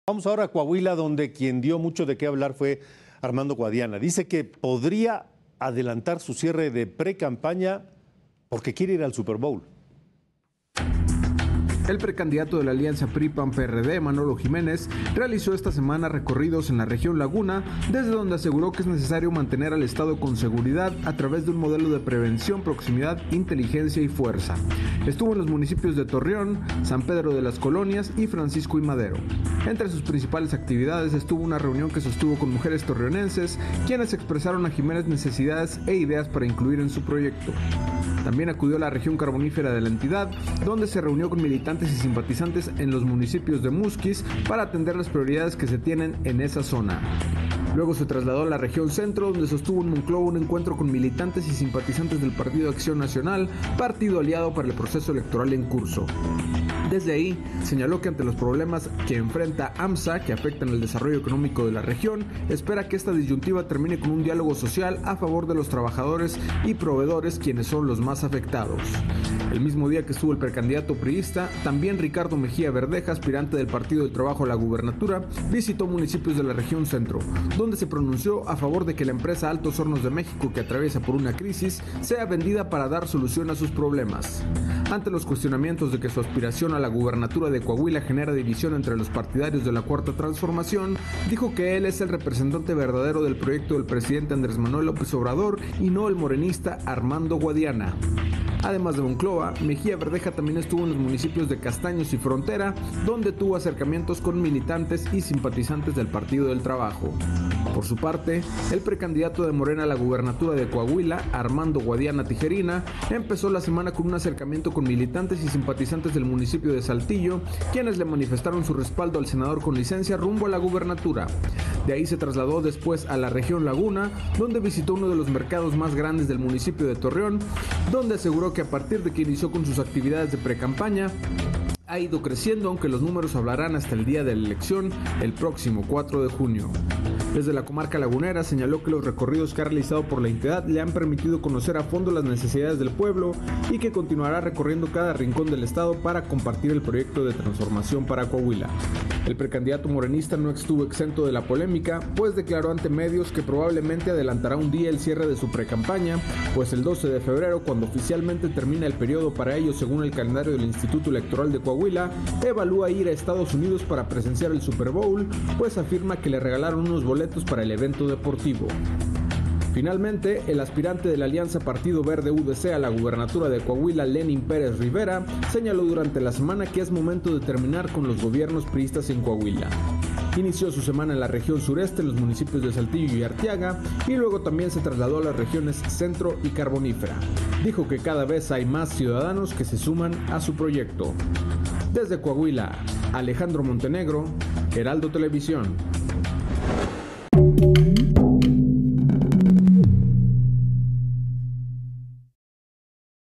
Elecciones Coahuila 2023: Armando Guadiana anticipará cierre de precampaña para ir al Super Bowl
El precandidato de Morena a la gubernatura de Coahuila, Armando Guadiana Tijerina, estuvo ayer en la frontera norte de Coahuila, donde aseguró que la precampaña que ha tenido está uniendo aún más a los simpatizantes del partido.

Cabe mencionar que anticipará el último día que tiene para la precampaña debido a que irá a Estados Unidos para disfrutar del Super Bowl.